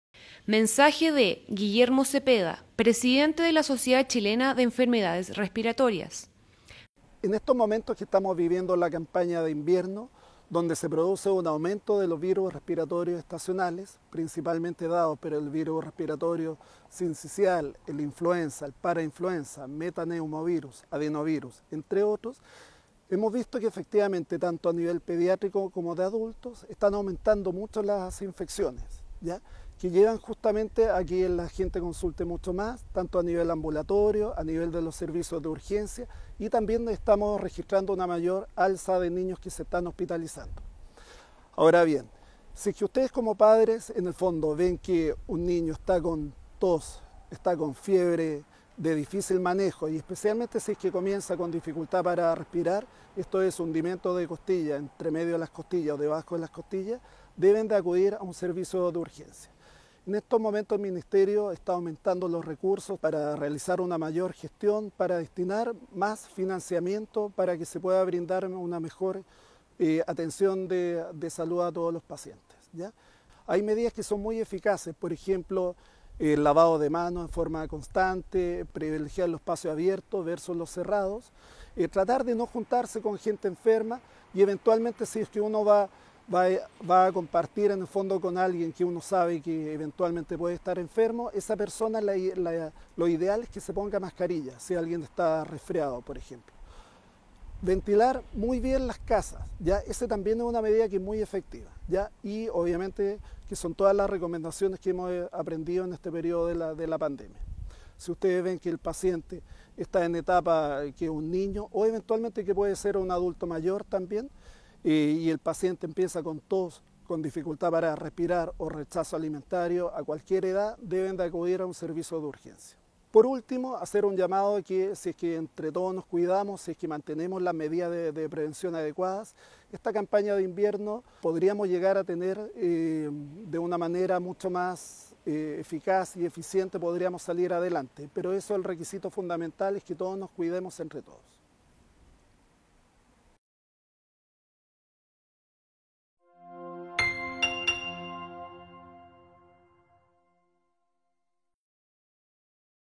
Medidas que se deben tomar por el aumento de las enfermedades respiratorias CampañaInvierno MINSAL
Dr. Guillermo Zepeda, Presidente de la Sociedad Chilena de Enfermedades Respiratorias - SER, nos informa las medidas que se deben tomar por el aumento de las enfermedades respiratorias en el marco de la CampañaInvierno del ministerio de Salud 🏥

🔴Asegúrate de seguir estas medidas para prevenir los contagios por enfermedades respiratorias:

➡️Lava de forma constante tus manos, si no dispones de agua potable, usa alcohol gel
➡️Evita espacios cerrados con aglomeraciones de personas
➡️Ventila al menos una vez al día tu hogar
➡️Si estas gestando o eres mayor a 65 años, vacúnate contra la influenza y mantén tu esquema completo contra el Covid_19